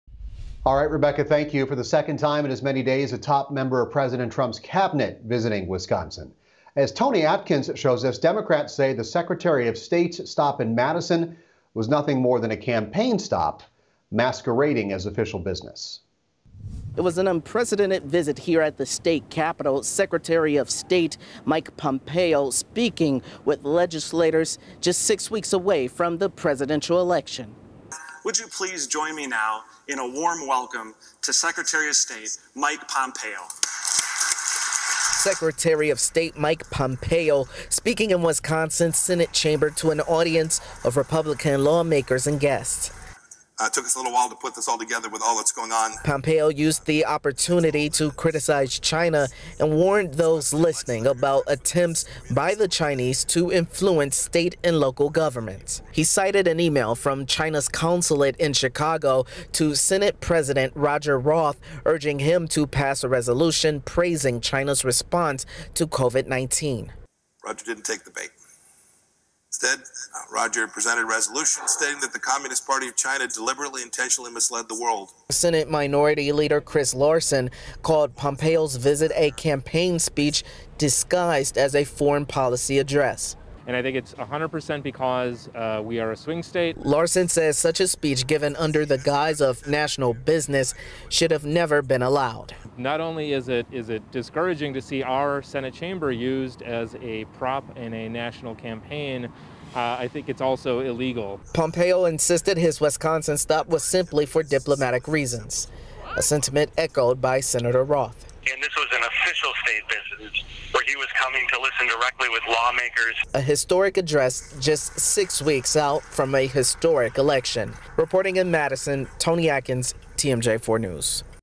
Pompeo warns of China influence in state, local governments
U.S. Secretary of State Mike Pompeo warned Wednesday that China was aggressively attempting to influence state and local governments, including through seemingly innocuous sister-city agreements, delivering the message in the presidential battleground state of Wisconsin.